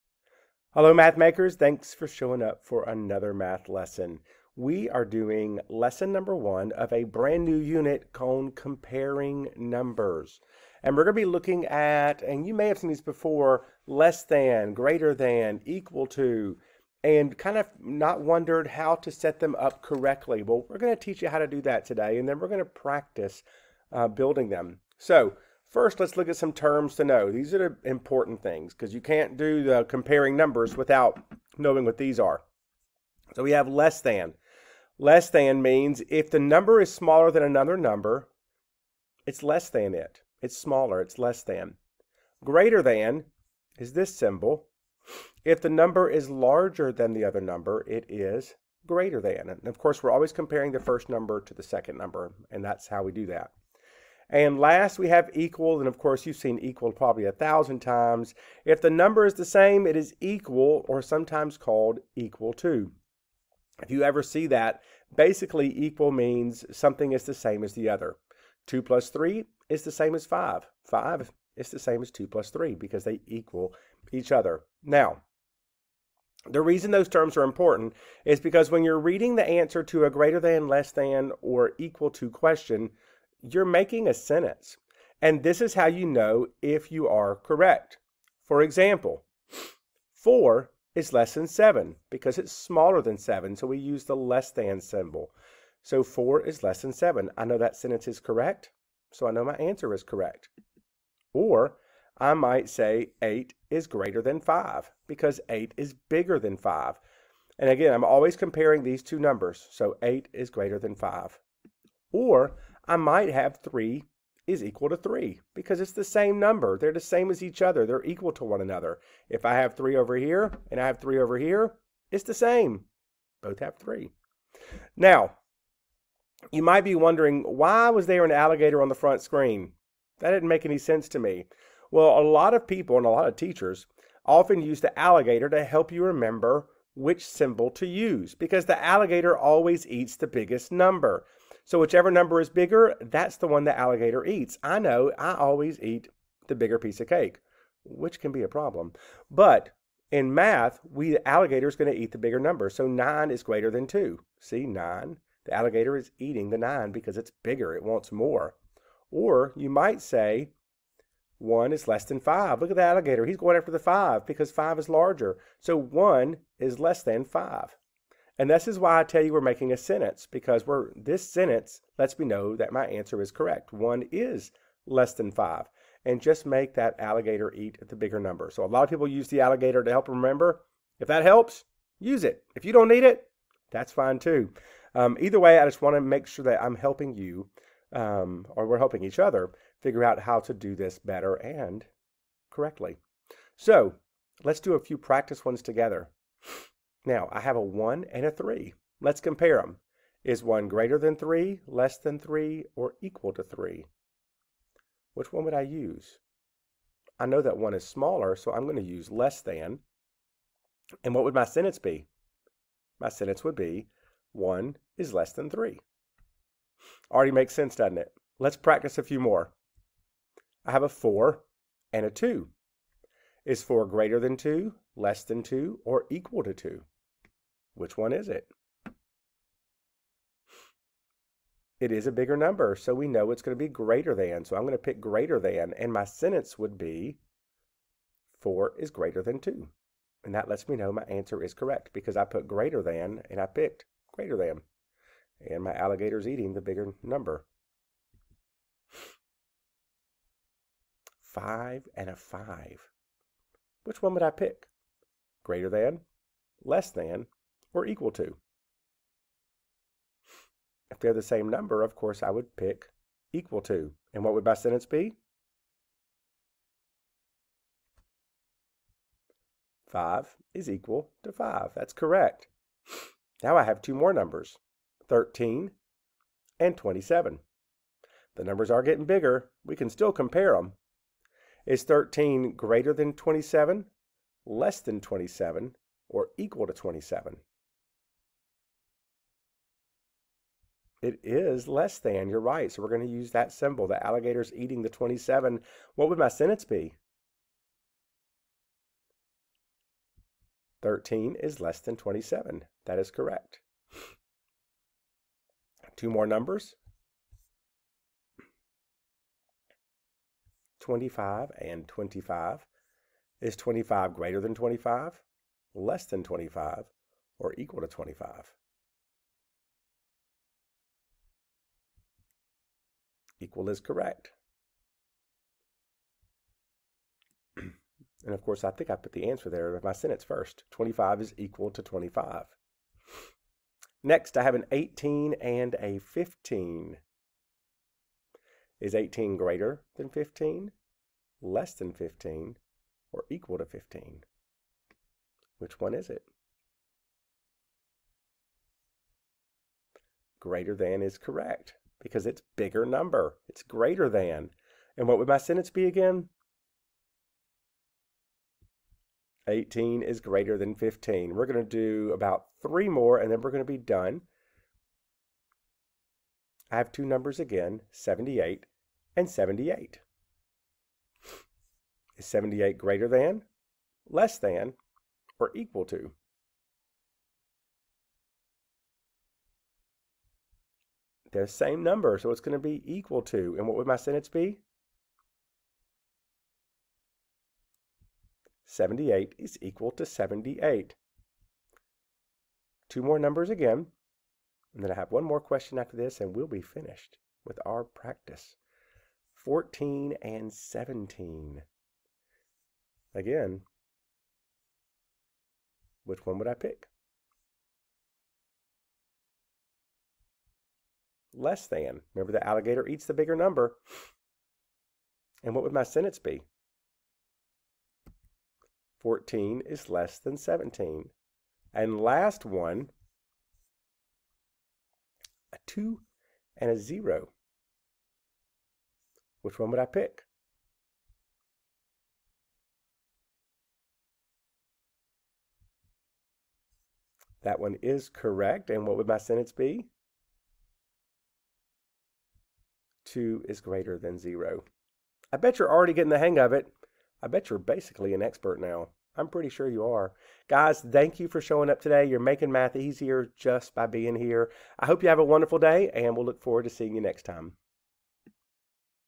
Comparing Numbers Greater Than, Less Than, Equal To - Lesson 1 (FULL LESSON)
In this lesson, we will begin a unit looking comparing numbers using greater than, less than, or equal to.

This is the full version of the previous video. For some reason, one a portion of the video posted.